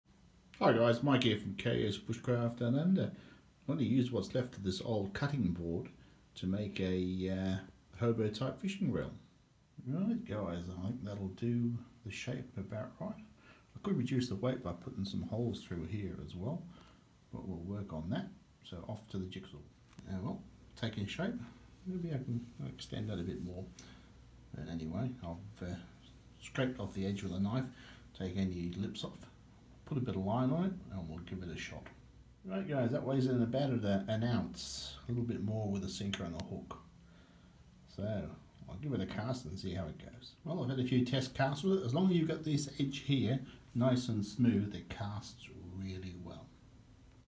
Making a hobo fishing 🎣 reel from a old cutting board
A simple DIY project